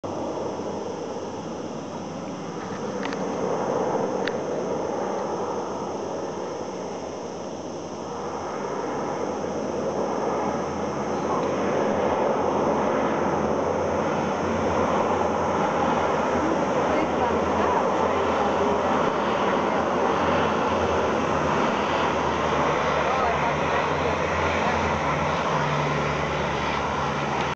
Blusvlietuig bij het meer van Istan (Marbella Spanje)
Dit vliegtuig haalt water uit het stuwmeer bij het dorpje Istan in Spanje